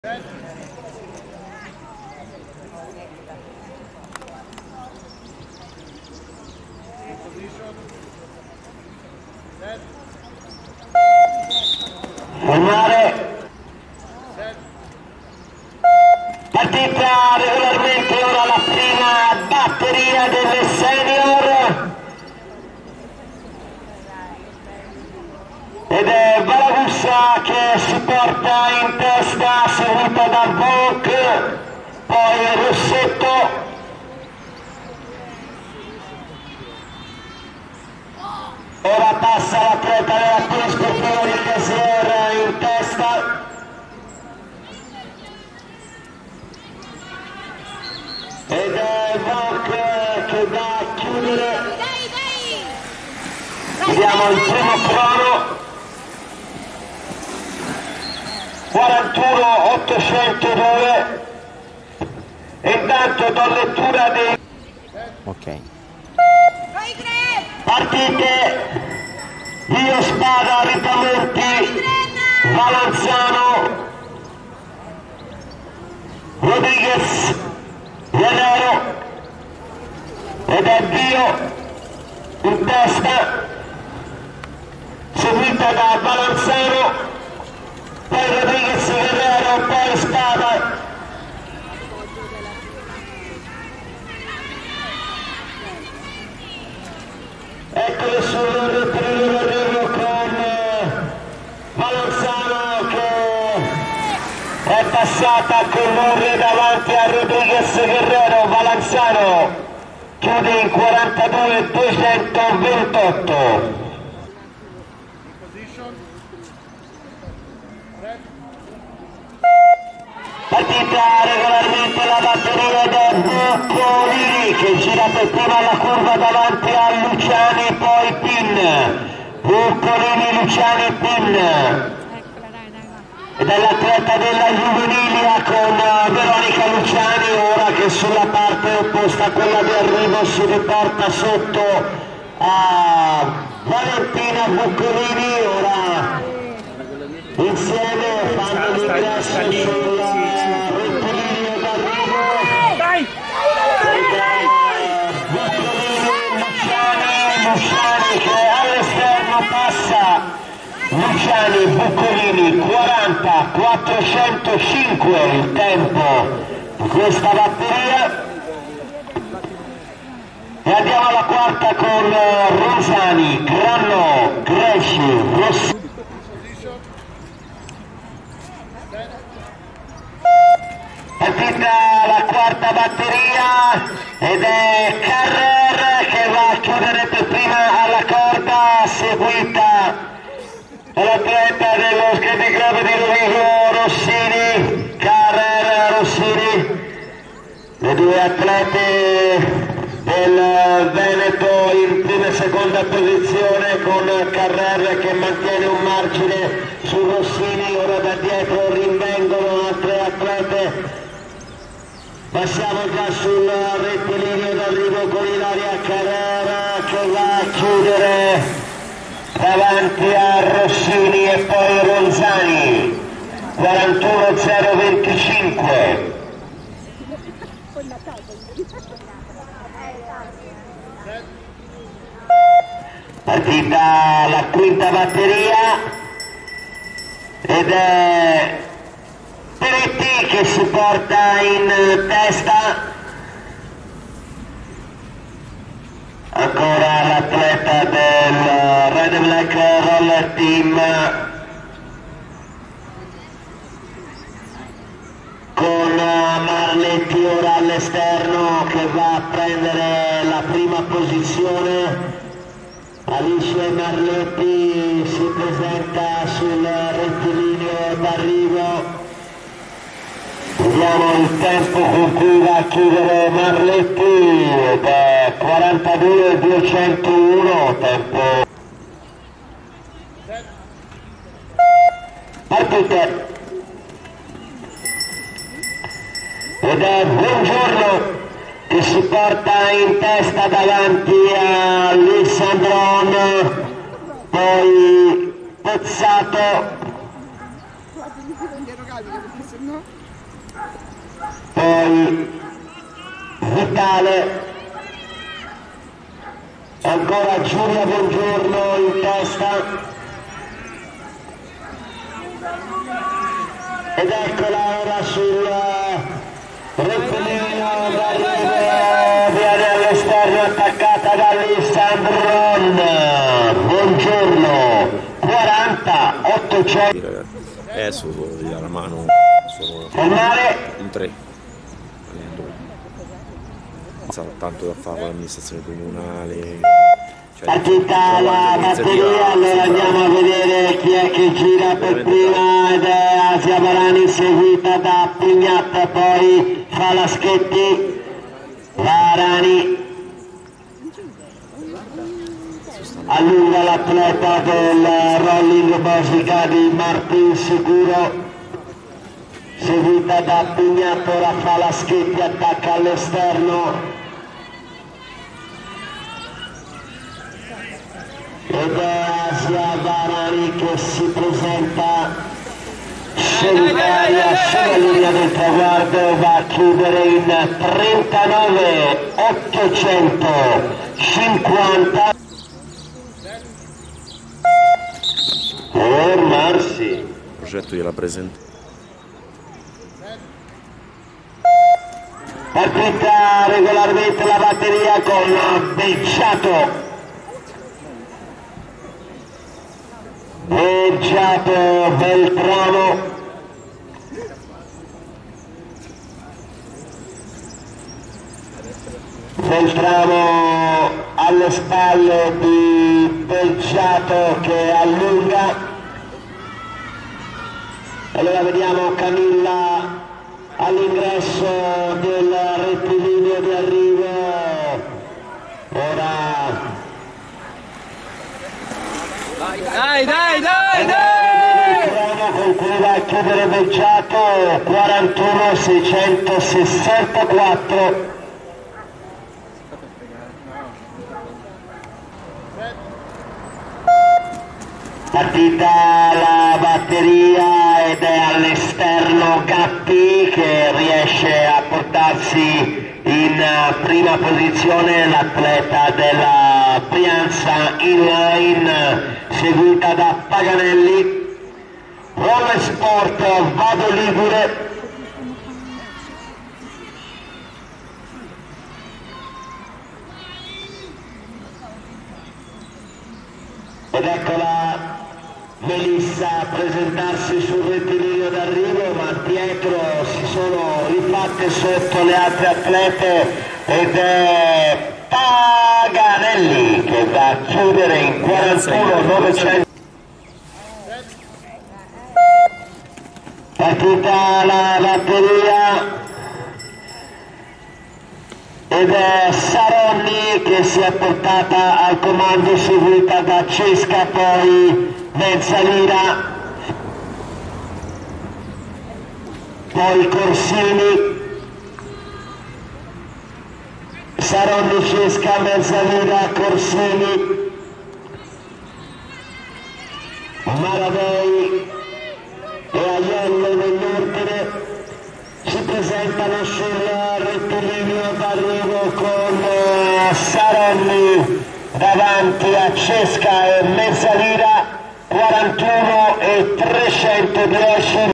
Campionati Italiani Strada 2023 JS - S/F 1 Giro Sprint, qualifiche
Campionati Italiani Pattinaggio Corsa 2023, specialità "strada".
Categorie Juniores e Seniores.
14 Maggio 2023, San Bendetto del Tronto (AP) - Italy.
1 giro sprint, qualifiche.

00:00 batteria 1, f.p.
00:13 batteria 1
01:04 batteria 2
01:53 batteria 3
02:55 batteria 4
03:48 batteria 5
04:38 batteria 6
05:26 batteria 7, f.p.
05:34 batteria 7
06:22 batteria 8, f.p.
06:29 batteria 8
07:22 batteria 9
08:10 batteria 10

1 241 VONK NORALY BERBER 2107 POL.VA CASIER ARL 41.786
2 90 VALAGUSSA REBECCA 1220 ROTELL.ROSEDA MERATE 42.276
3 851 ROSSETTO ELENA 2733 AZZURRA PATT. 44.380
4 737 BIAGI SOFIA 3809 A.S.D.BRIANZA INLINE 45.973
5 228 FAGGION CLAUDIA 1290 SKATING RHO 46.099

Falsa partenza del n. 851 ROSSETTO ELENA

1 410 VALANZANO GIORGIA 302 S.S.D.S.MENS SANA 42.211
2 495 RODRIGUEZ GUERRERO YINETH STEFANNY 785 GS SCALTENIGO VE 42.662
3 34 VIO CHIARA 752 PATT.SPINEA 43.301
4 52 SPADA YASMINE 91 A.H.P. PADOVA ASD 43.707
5 89 RIPAMONTI GRETA 1220 ROTELL.ROSEDA MERATE 44.761

1 43 LUCIANI VERONICA 785 GS SCALTENIGO VE 40.387
2 240 BUCCOLINI VALENTINA 1179 A.P.D.JUVENILIA 40.591
3 87 PIN MATILDE 1220 ROTELL.ROSEDA MERATE 44.110
4 261 ACETI SELENE 1136 SKATING C.UBOLDO 45.703
5 127 VERZENI GIORGIA 4185 POL. CALUSCHESE 45.939
6 264 CADENAZZI RAFFAELLA 651 G.P.MOBILI CANTU' 52.761

1 130 CARRER ILARIA 2107 POL.VA CASIER ARL 41.009
2 185 ROSSINI BENEDETTA 98 SKATING C.ROVIGO 41.475
3 140 RONZANI ILARIA 785 GS SCALTENIGO VE 41.551
4 148 GRECI ERICA 836 FORTITUDO FABRIANO 41.748
5 808 GRANNO' MICHELLE 2386 PATT. SAMBENEDETTESI 43.360
6 821 FASAN VANESSA 3754 NEW R.PORCIA ASD 45.436

1 142 MARLETTI ALICE 1285 PATT.S.MAURO TO 42.184
2 741 PERETTI CLAUDIA 3816 REDBLACK R.TEAM 42.692
3 849 NACCARI IRENE 3463 I FALCHI 42.895
4 816 CORSINI GIORGIA 3160 CITTA' DI P.GARGALLO 43.089
5 760 FERRETTI MARTINA 4011 LUNA SPORTS ACADEMY 44.230
199 LOLLOBRIGIDA GIULIA 2590 CSEN ROMA PATT. NP

1 745 LISSANDRON GAIA 98 SKATING C.ROVIGO 40.883
2 133 BONGIORNO GIULIA 173 S.CLUB COMINA 40.965
3 201 POZZATO FRANCESCA 752 PATT.SPINEA 41.279
4 37 VITALE LUCREZIA 2564 SAVONA IN LINE 41.512
5 803 CESARO EMMA 785 GS SCALTENIGO VE 42.721
6 791 MASSARA CLAUDIA 4000 ASD INLINE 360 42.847

1 476 VARANI ASJA 1260 ROLLING BOSICA 39.834
2 268 DE PALMA LUCREZIA 302 S.S.D.S.MENS SANA 41.686
3 93 FALASCHETTI IRENE 2386 PATT. SAMBENEDETTESI 41.712
4 419 PARMA REBECCA 310 POL. BELLUSCO 42.043
5 125 PIGNAT CHIARA 173 S.CLUB COMINA 42.333
6 136 BERTO ELISA 98 SKATING C.ROVIGO 42.794

Falsa partenza del n. 93 FALASCHETTI IRENE

1 129 BEGGIATO CAMILLA 2107 POL.VA CASIER ARL 41.649
2 758 TAGLIATA ROBERTA MARIA 3160 CITTA' DI P.GARGALLO 44.016
3 139 MEUCCI MARGHERITA 302 S.S.D.S.MENS SANA 44.099
4 232 GIROLDI ANNA 173 S.CLUB COMINA 44.100
5 766 BELTRAMO REBA ANGELA 1285 PATT.S.MAURO TO 44.189
6 850 CADENAZZI MOIRA 651 G.P.MOBILI CANTU' 49.257

1 81 PAGANELLI IRENE 4333 R.S.VADO LIGURE ASD 41.884
2 757 GATTI MELISSA 3809 A.S.D.BRIANZA INLINE 41.911
3 85 BELLANI GIULIA 1220 ROTELL.ROSEDA MERATE 42.081
4 21 BORMIDA GIORGIA 302 S.S.D.S.MENS SANA 42.479
5 769 CANAZZA ILARIA 91 A.H.P. PADOVA ASD 43.725
244 DI NATALE FEDERICA 1091 C.P.G.A. ASD NP

1 838 SARONNI SOFIA 310 POL. BELLUSCO 41.296
2 61 CESCA ALESSIA 752 PATT.SPINEA 41.554
3 749 MEZZALIRA ALESSIA 2733 AZZURRA PATT. 41.810
4 751 CORSINI GIULIA 3816 REDBLACK R.TEAM 42.014
5 506 MARADEI CHIARA 4000 ASD INLINE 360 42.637
6 73 AIELLO GIORGIA 3370 CARDANO SKATING 44.401